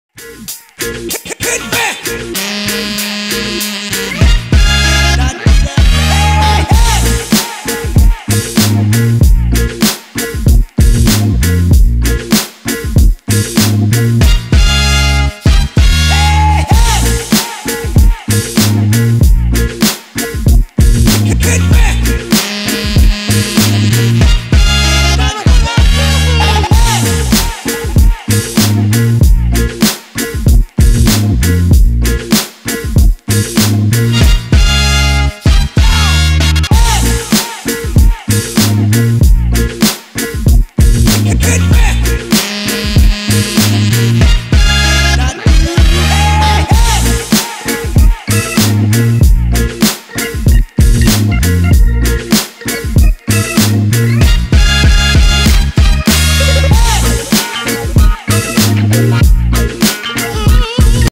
Mr. Jackson vs. Insane Creators Trainvideo
Short video from the train to show you guys my latest wall! 🙌🏻 🎥 🎧 🚂